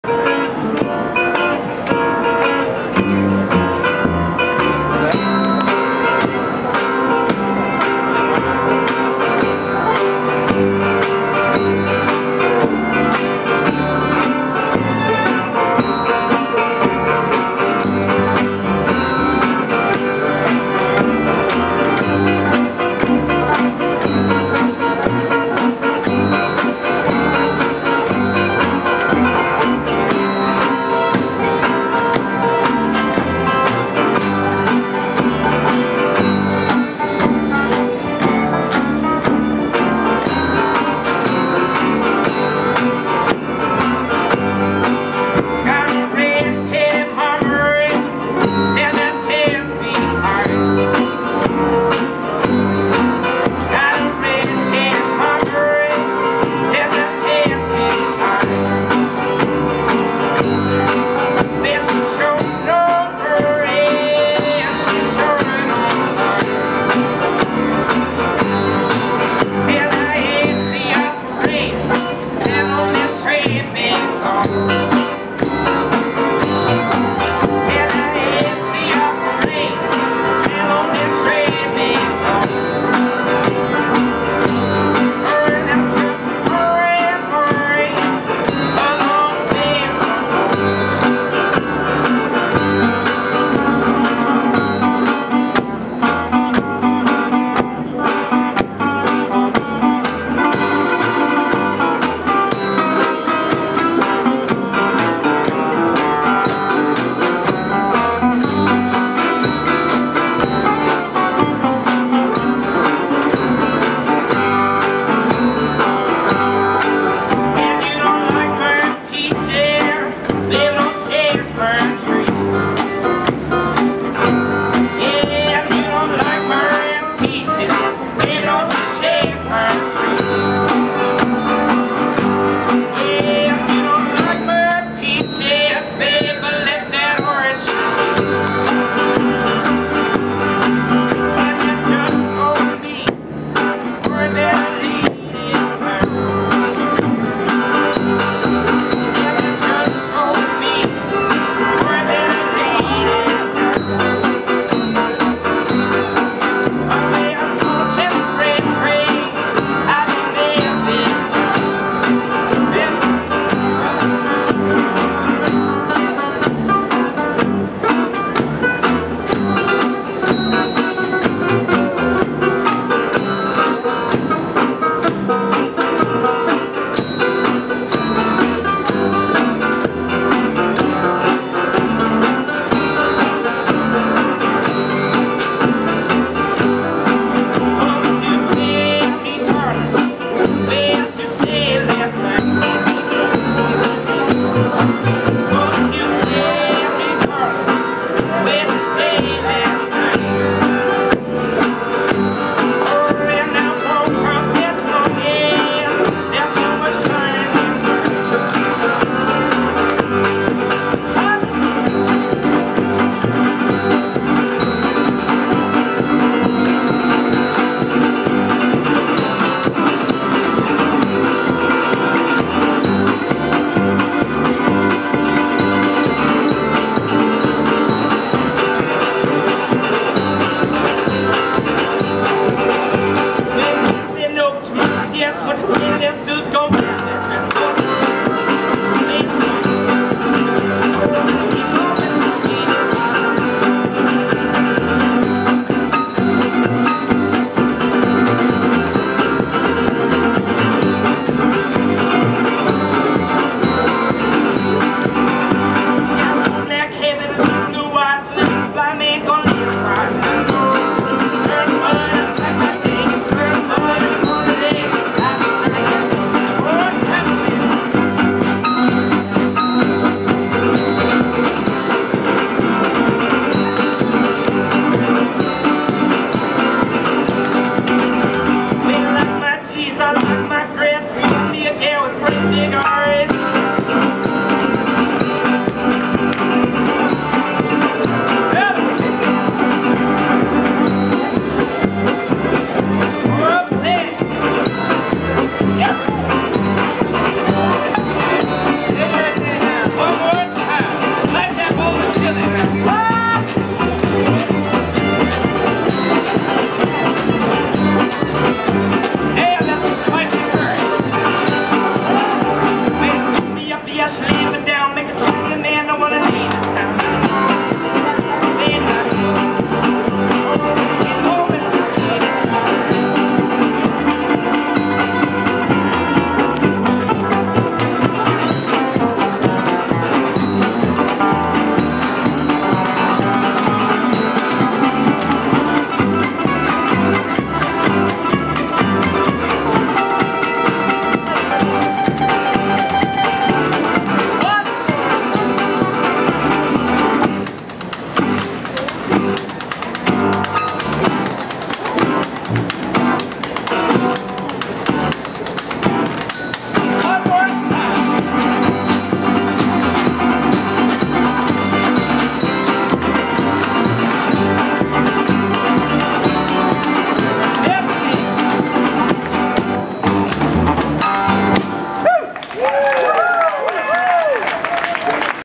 Yes Ma'am on Royal Street in New Orleans
The latest edition of the ever popular Yes Ma'am is the basic trio plus banjo, bass, and some excellent resonator guitar work from Shine Delphi. Recorded April 18, 2014